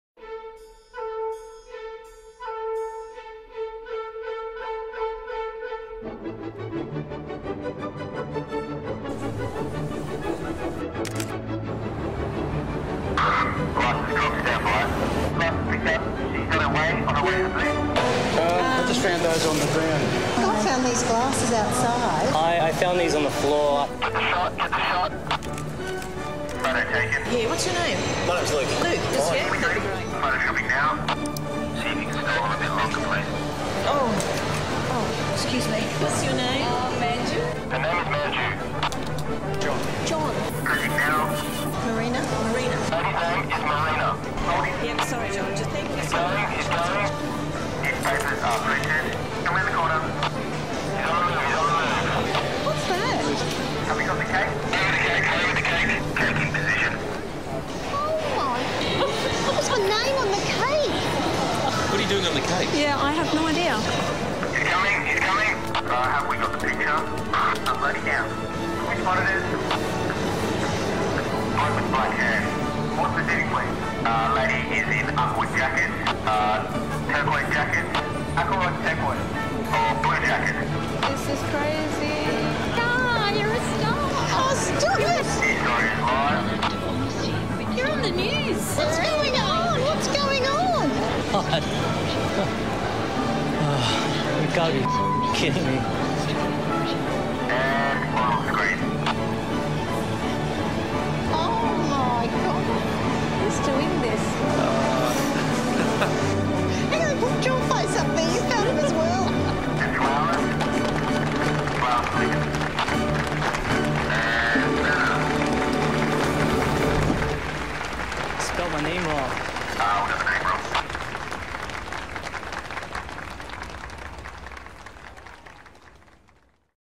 L'Honnêteté Devrait Être Toujours Récompensée !
Une caméra cachée avec les meilleures intentions : on laisse traîner une paire de lunettes de soleil devant le magasin et on attend de voir qui a l'honnêteté de les ramener à l'accueil.. Sauf qu'il ne s'agit pas d'un vrai accueil et qu'en moins de cinq minutes, cet acte honnête devient un événement national ! Excellent !